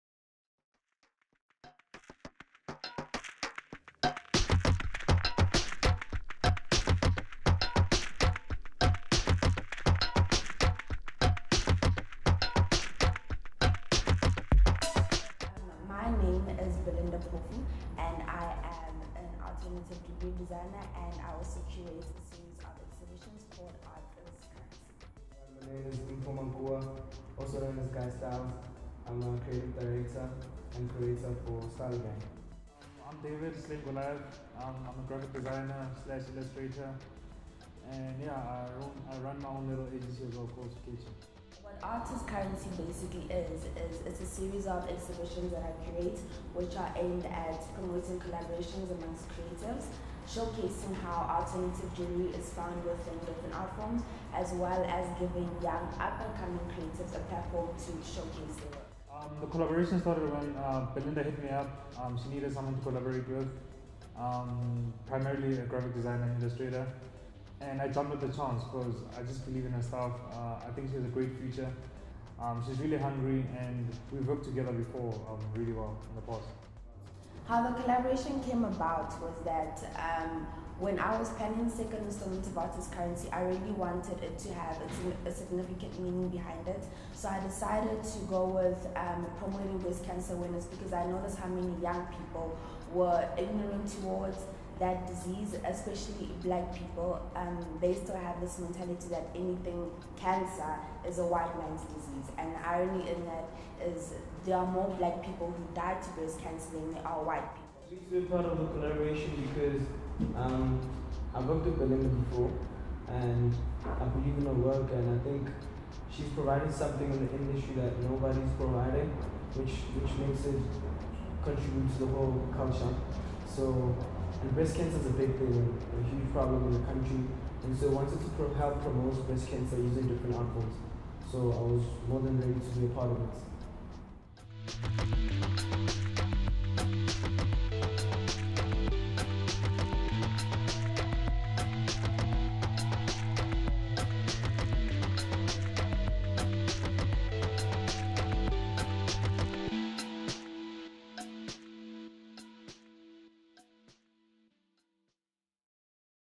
BLACKNATION VIDEO NETWORK presents SIIC, ART IS CURRENCY Promo Video
We’ve pushed boundaries when it comes to collaborations with the 2nd instalment of Art Is Currency. And we hope we inspire you guy’s to do this same.

So much work has been going into this project.I can not wait for the big day.

…13 more days to go guy’s :)